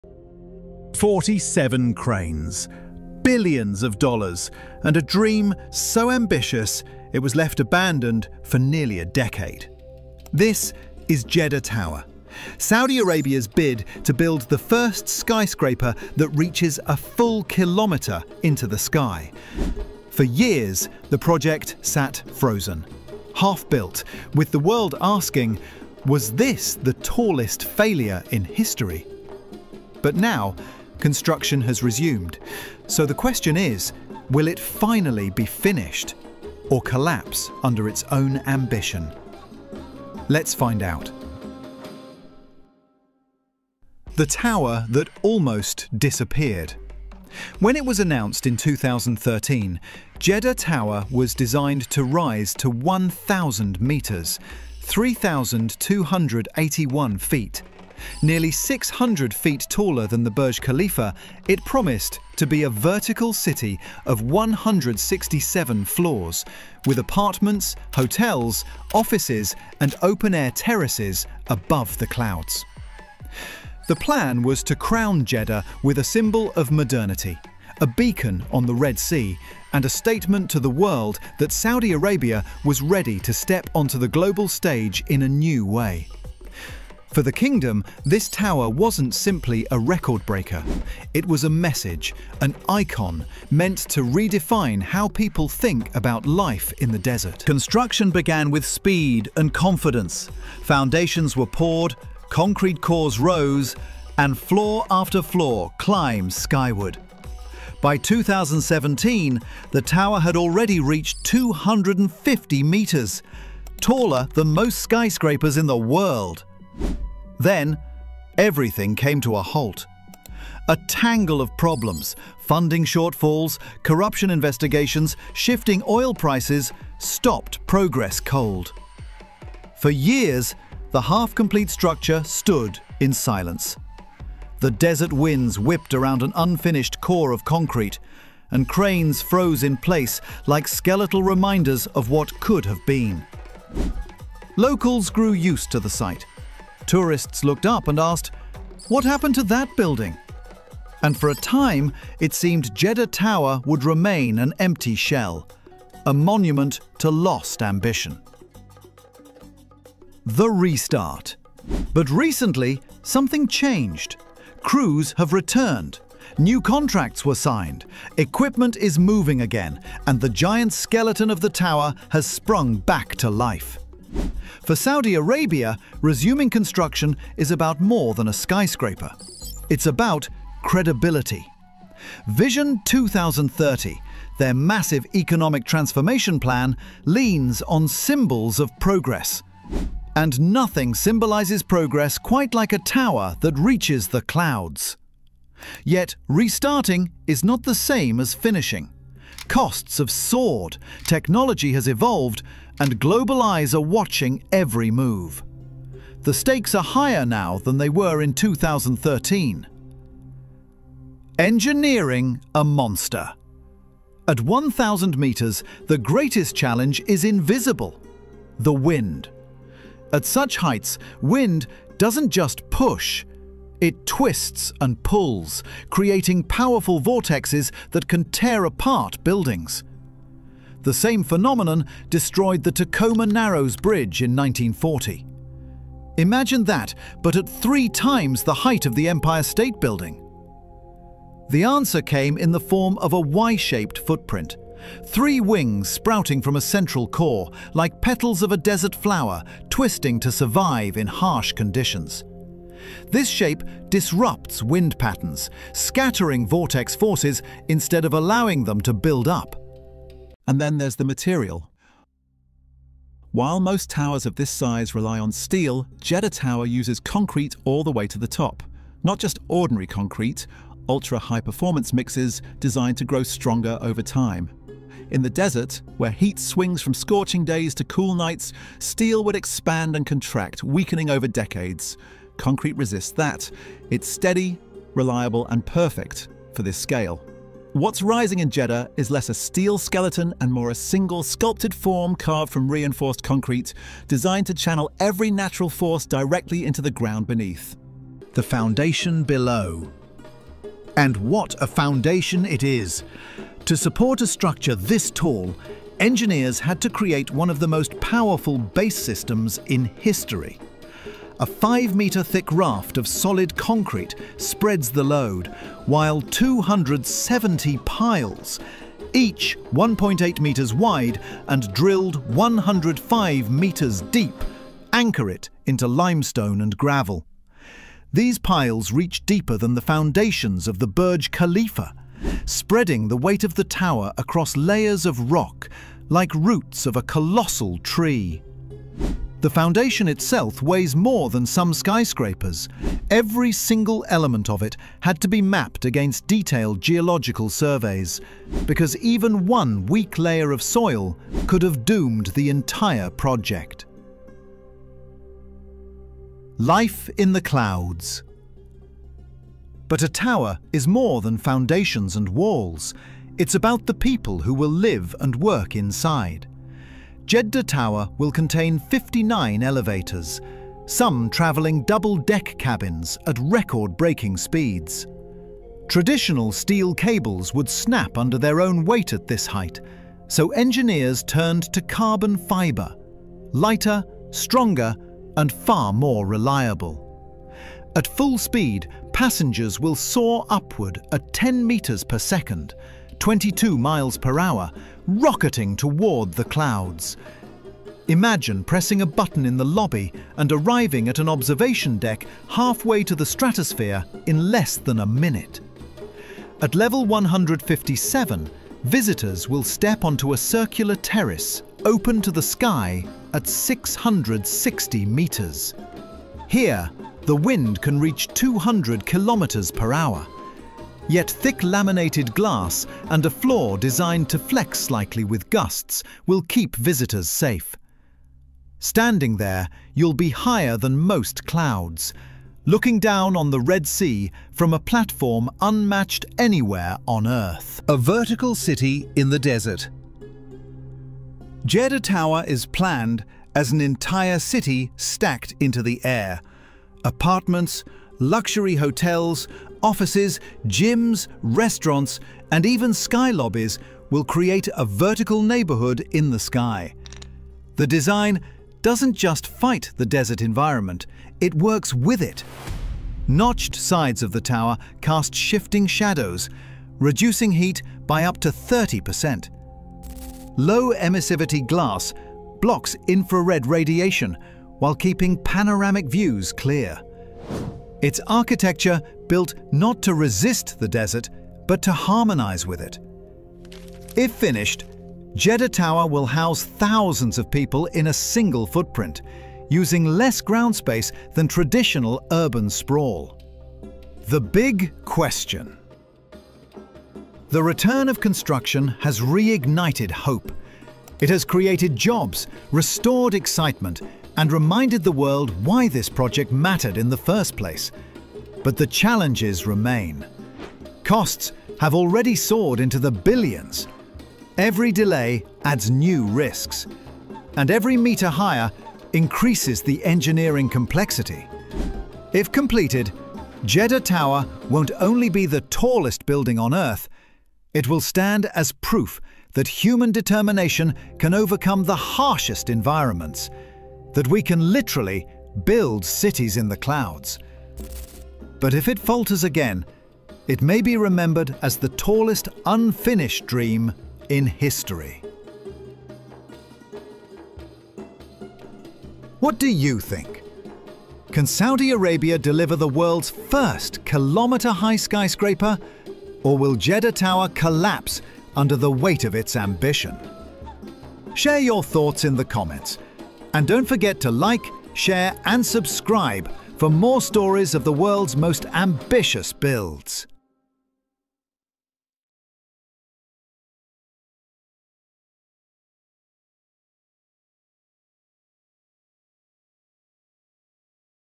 Jeddah Tower: The 1km Tower Racing Past Burj Khalifa
Explore the story of the Jeddah Tower, a massive saudi arabia skyscraper that aimed to reach unprecedented heights as one of the most ambitious megaprojects. Despite the initial vision 2030 saudi arabia goals and significant construction efforts, the project faced unexpected hurdles, leaving many to question the future of its architecture and engineering.